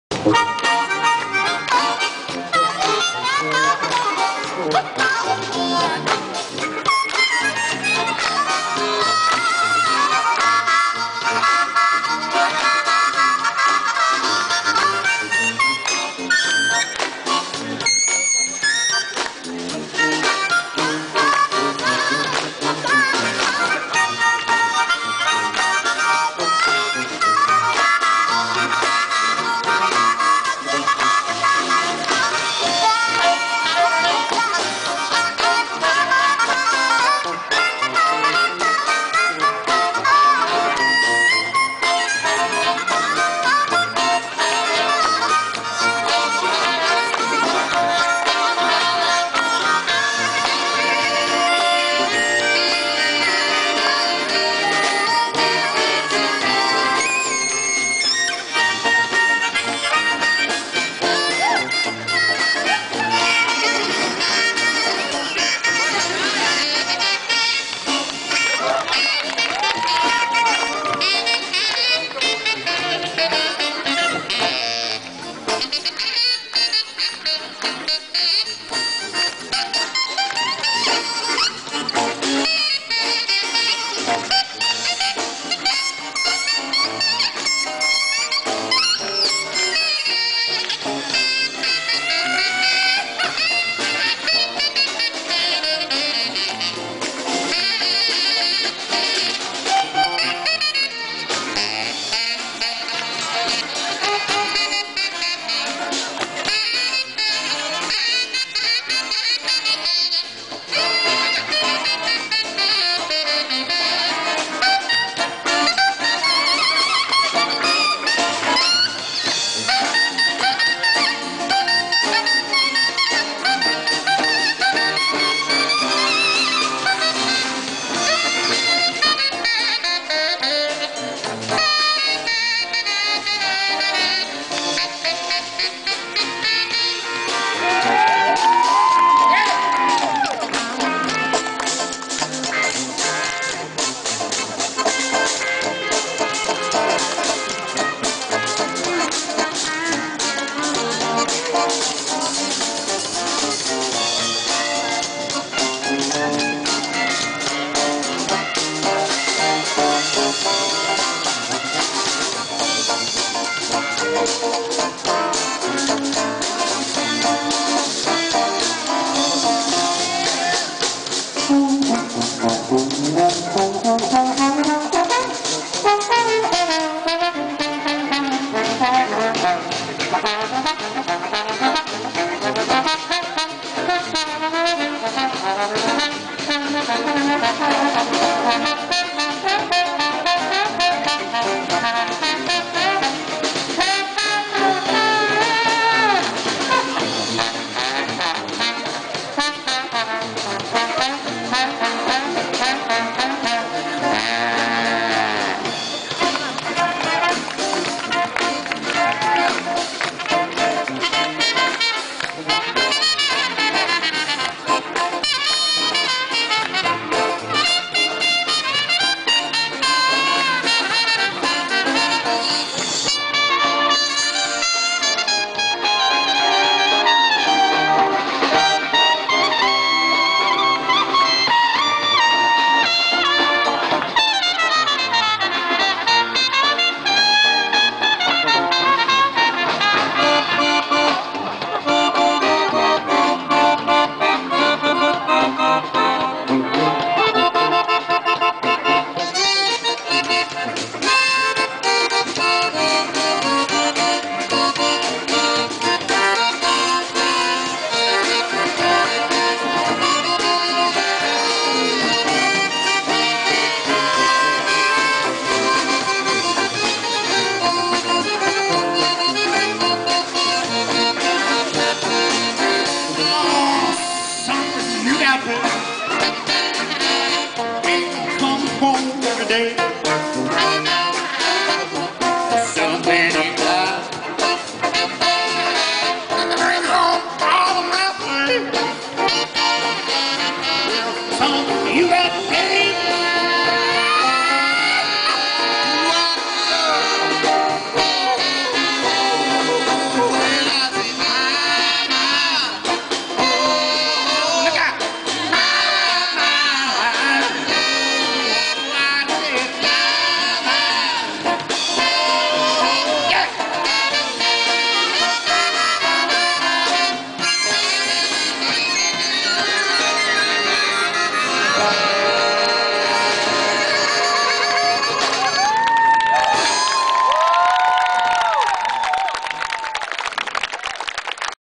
Hazmat Modine Live in Kazan
Выступление команды Hazmat Modine (США) в Казани (Республика Татарстан, Россия) на ул. Баумана около памятника Шаляпину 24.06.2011, за день до фестиваля "Сотворение мира" на благотворительном концерте, посвященном сбору пожертвований для нуждающихся музыкантов.